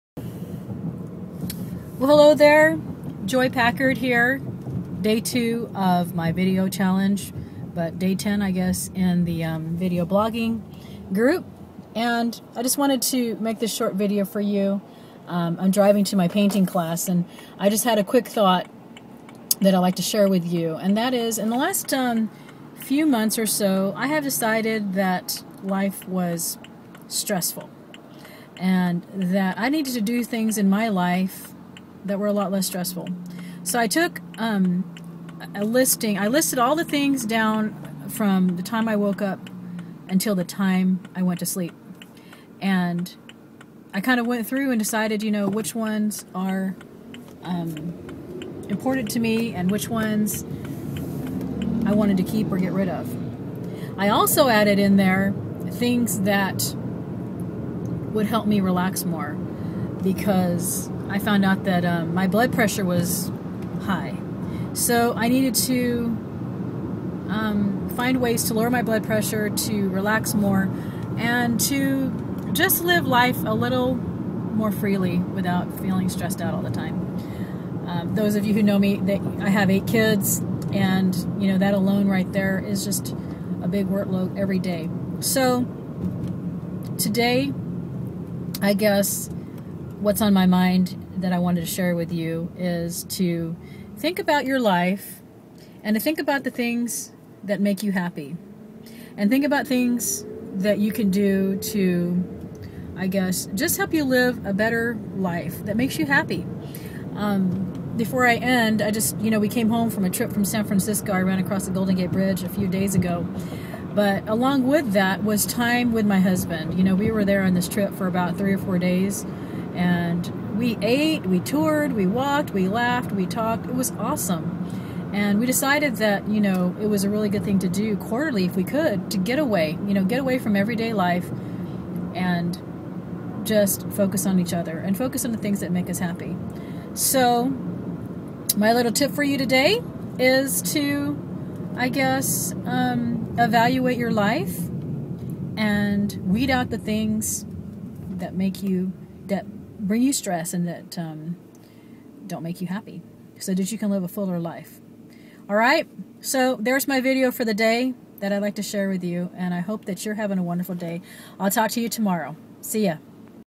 Destress, Be Happier!
Sometimes we just go through life day in and day out...all stressed out!  There is so much more out there to enjoy!